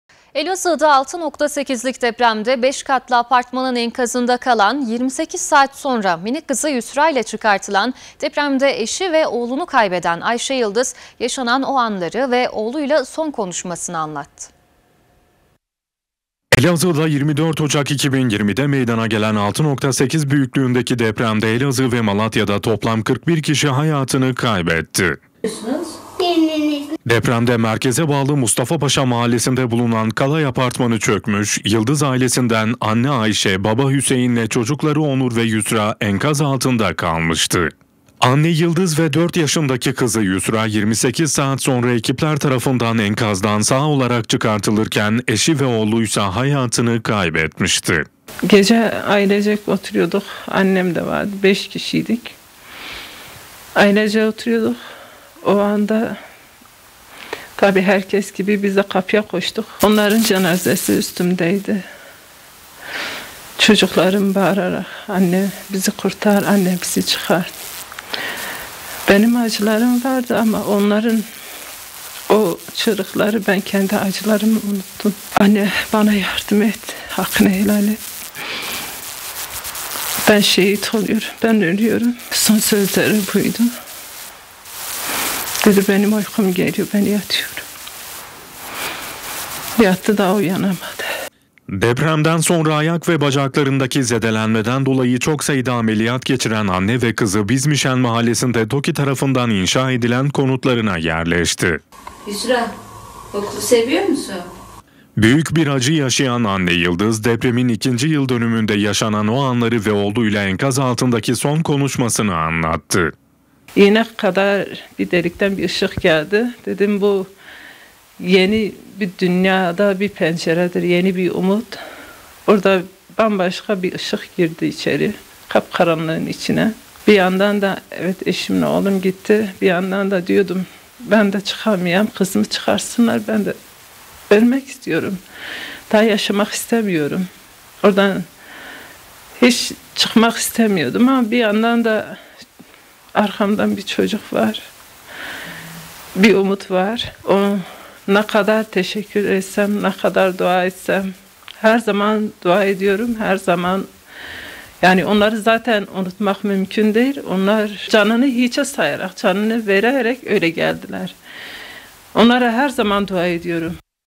DEPREMDE EŞİNİ VE OĞLUNU KAYBETTİ
ELAZIĞ’DA 6.8’LİK DEPREMDE 5 KATLI APARTMANIN ENKAZINDA KALAN, 28 SAAT SONRA MİNİK KIZI YÜSRA İLE ÇIKARTILAN, DEPREMDE EŞİ VE OĞLUNU KAYBEDEN AYŞE YILDIZ, YAŞANAN O ANLARI VE OĞLUYLA SON KONUŞMASINI ANLATTI.